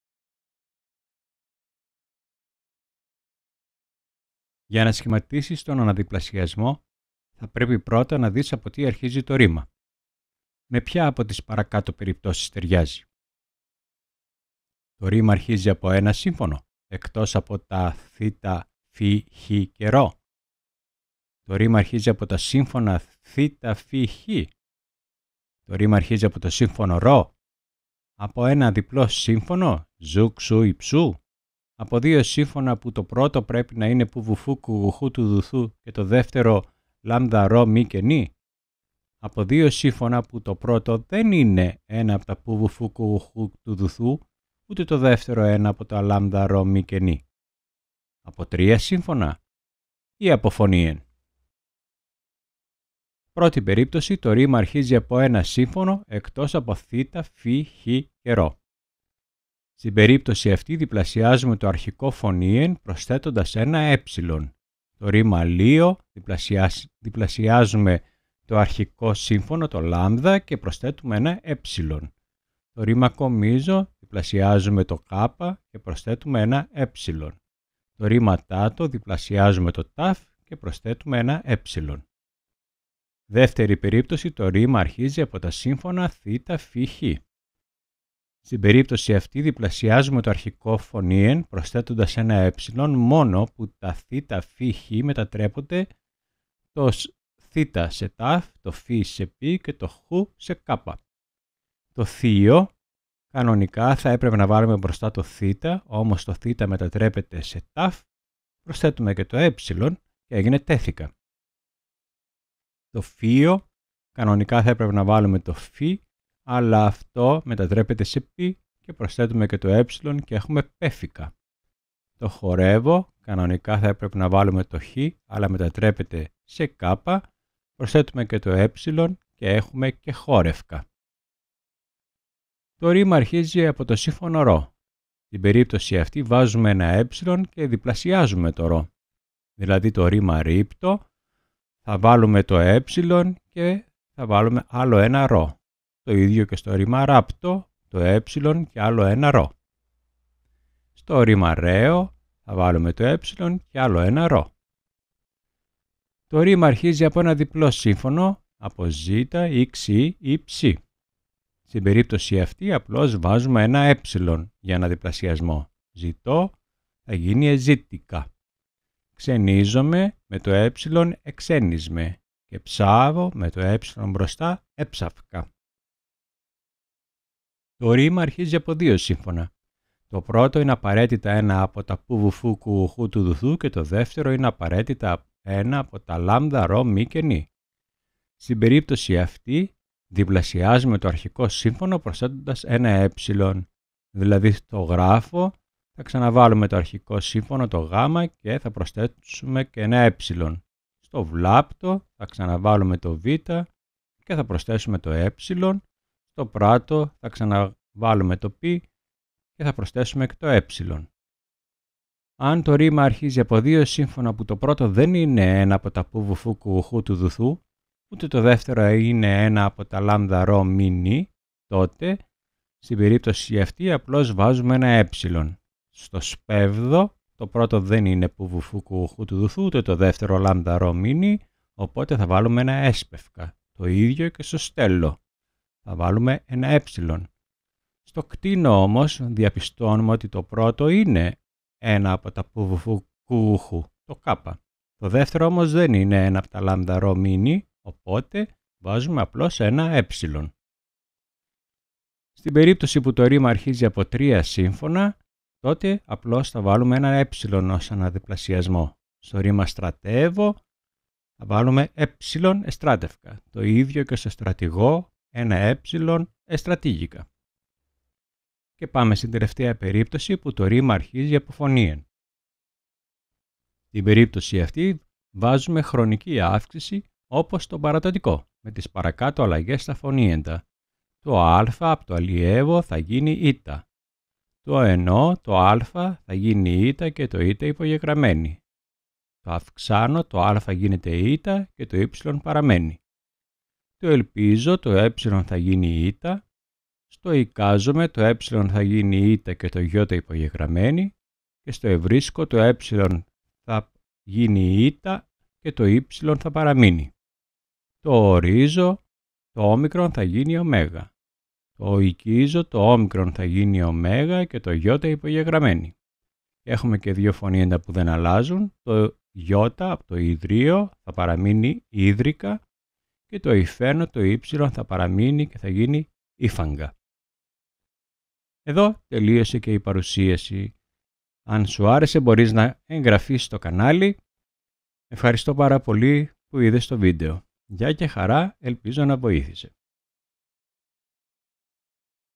Οδηγίες για τον αναδιπλασιασμό του παρακείμενου στα αρχαία ελληνικά.
Περιεχόμενα:
0:52 1. από ένα σύμφωνο (εκτός από: θ, φ, χ, ῥ)
01:23 2. από τα σύμφωνα θ, φ, χ
02:12 3. από το σύμφωνο ῥ
02:36 4. από ένα διπλό σύμφωνο (ζ, ξ, ψ)
02:56 5. από δύο σύμφωνα (το 1ο π, β, φ, κ, γ, χ, τ, δ, θ το 2ο λ, ρ, μ, ν
03:32 6. από δύο σύμφωνα (το 1ο δεν είναι: π,β,φ,κ,γ,χ,τ,δ,θ ούτε το 2ο λ,ρ,μ,ν
04:12 7. από τρία σύμφωνα
04:28 8. από φωνήεν